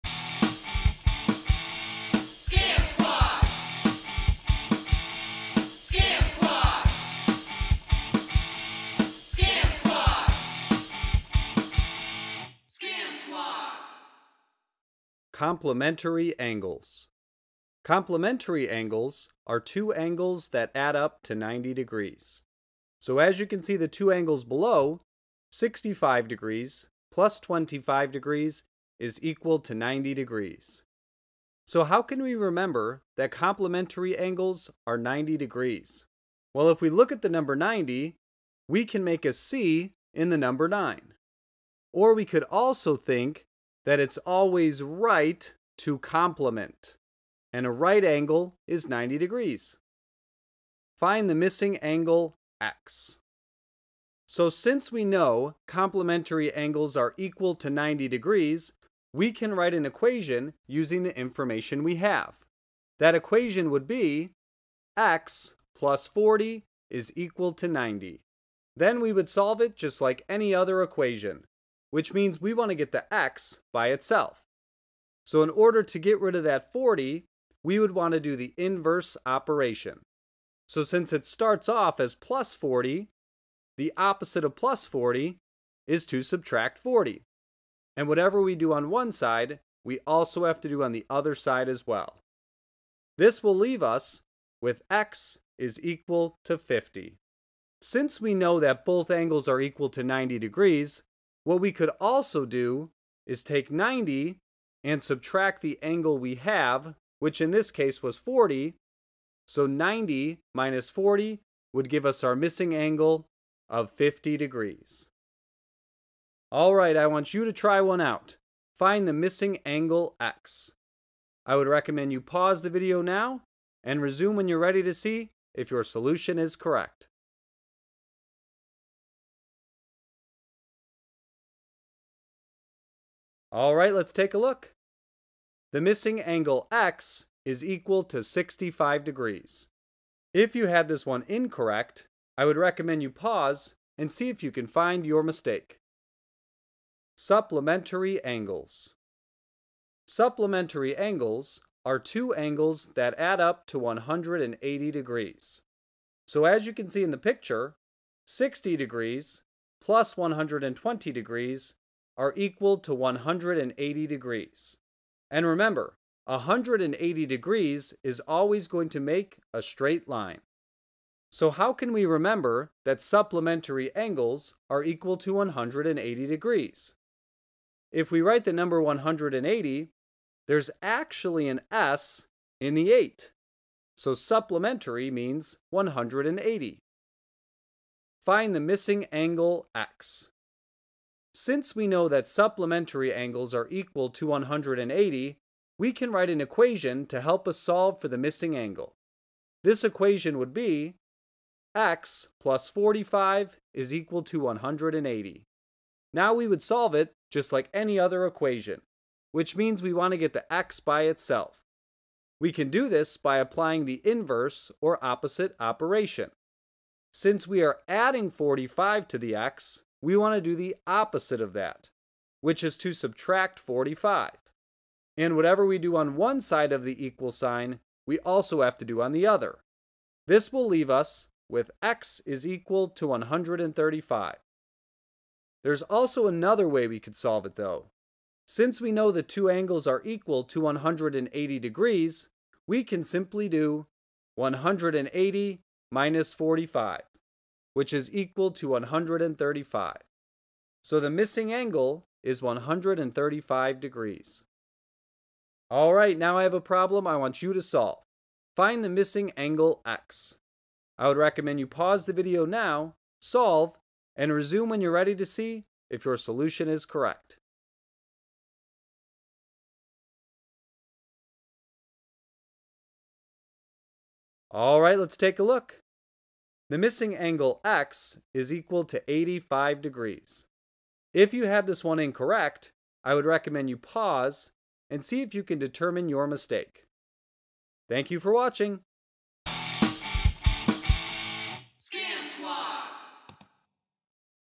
Complementary and Supplementary Angles (Find the Missing Angle)
This video teaches viewers about complementary and supplementary angles. The video includes the definitions of complementary and supplementary angles, example problems for how to find a missing complementary or supplementary angle, and practice problems for the viewer to try.